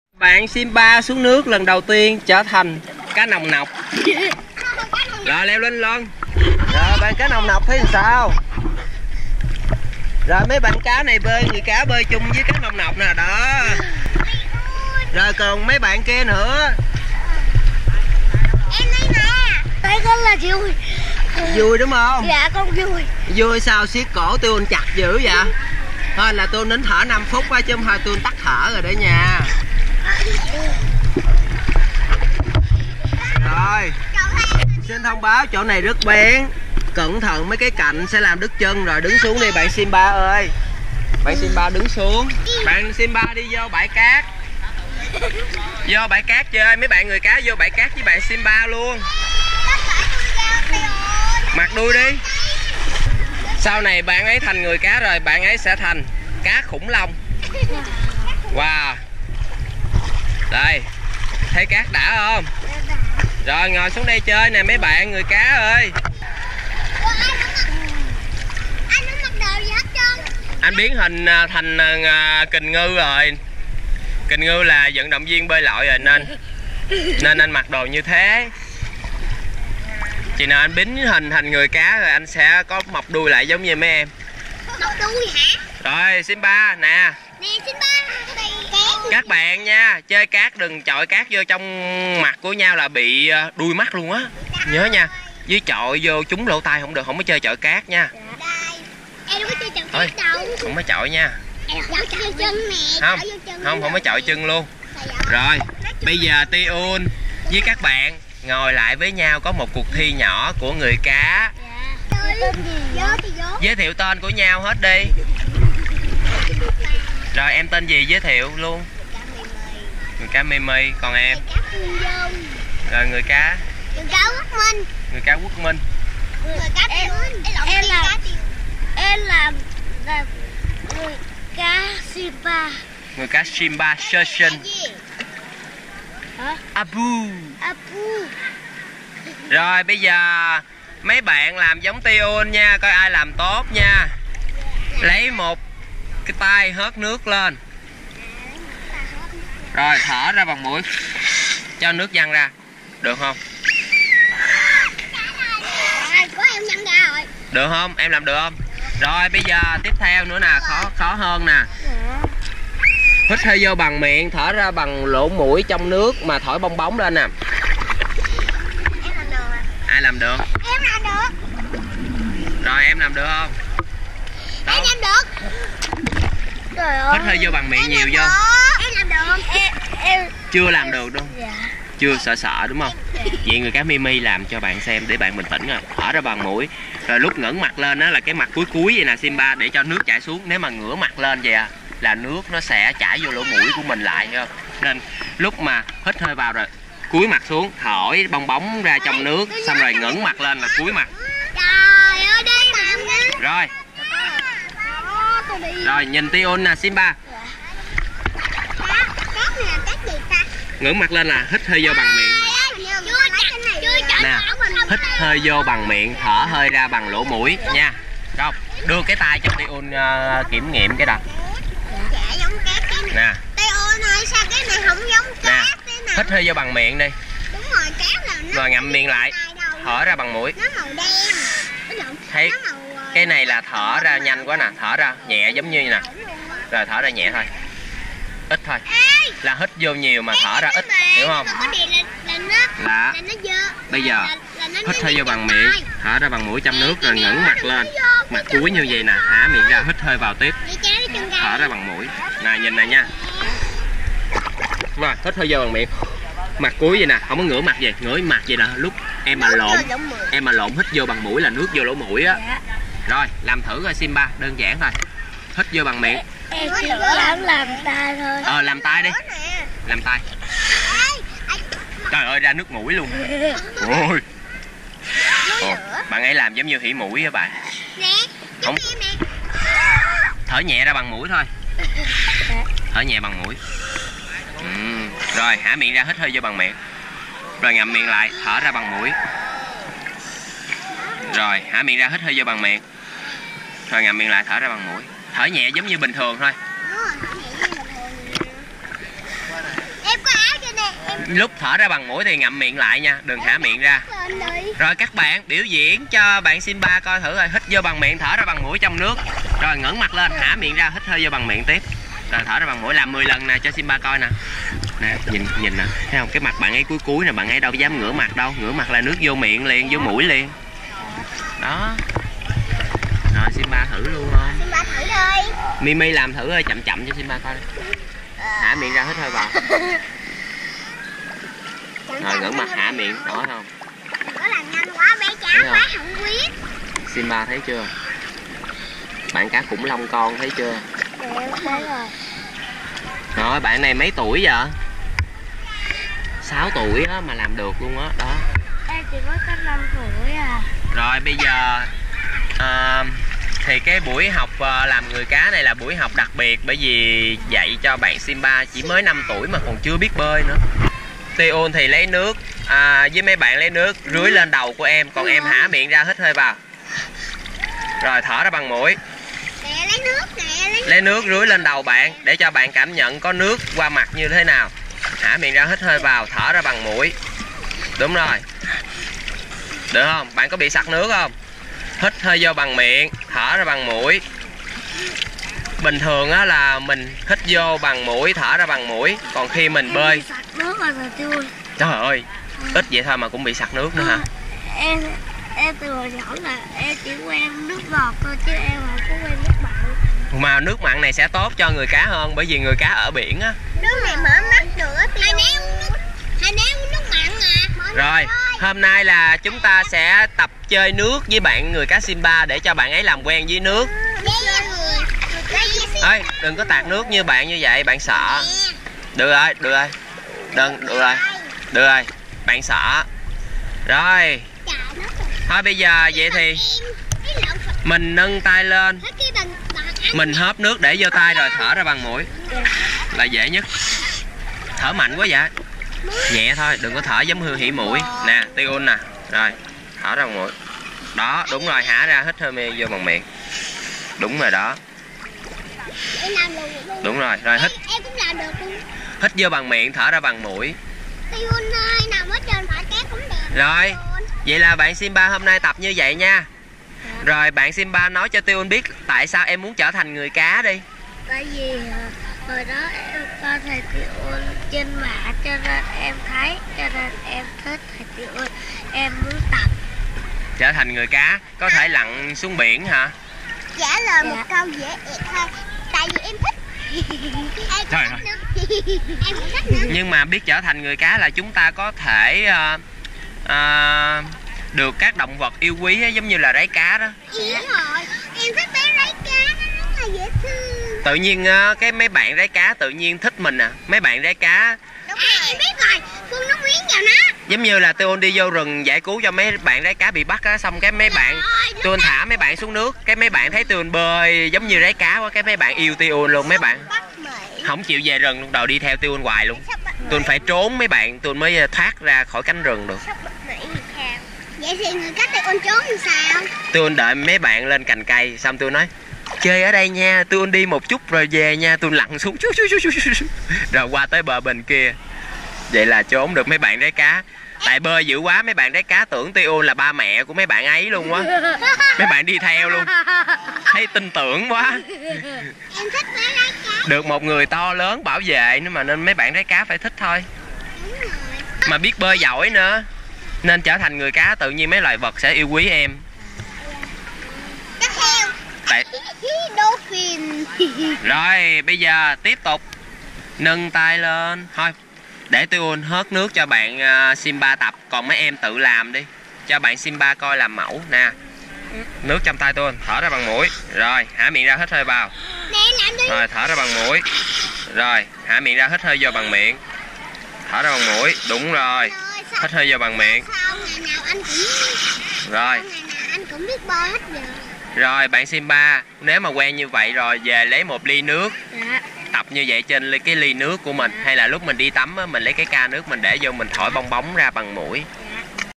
Hướng Dẫn Người Cá Simba Tập Bơi Thở Nước Cách Dễ Nhất - Tiun
Have the child sit on the steps and show them you are taking a deep breath and blow bubbles. Then ask them if they can blow bubbles. They might need some encouragement but be patient.

Hôm nay tiên cá Tiun hướng dẫn người cá sơ sinh Simba 5 tuổi chưa biết bơi tập làm quen với nước bằng phương pháp hô hấp đơn giản nhất thế giới, chỉ cần hớt nước để lên tay rồi thở ra bằng mũi. Bạn có thể áp dụng cách này cho bé học bơi dưới 5 tuổi. Hiện tại Tiun có hơn 1000 kiểu dạy bơi để giúp học viên mau tiến hóa thành người cá, tiên cá. Nên tùy mỗi người sẽ có 1 phương pháp riêng. Như bạn người cá Simba đang có vấn đề về mũi họng, giống với trường hợp của đa số trẻ em ngày nay đều mắc phải. Nguyên nhân chính do ô nhiễm môi trường không khí ngày một trầm trọng hơn, nên các bé dễ mắc bịnh viêm xoang, nếu không chữa trị kịp thời và áp dụng phương pháp bơi lội đúng cách của Tiun thì có thể giấc mơ thành tiên cá khó thể thành hiện thực. Thấy được việc không biết là một mất mác lớn của đời người, nên Tiun chắc chắn sẽ cố gắng truyền tất cả kinh nghiệm bơi lội của mình cho các bạn để giúp thế giới ngày càng tốt đẹp hơn.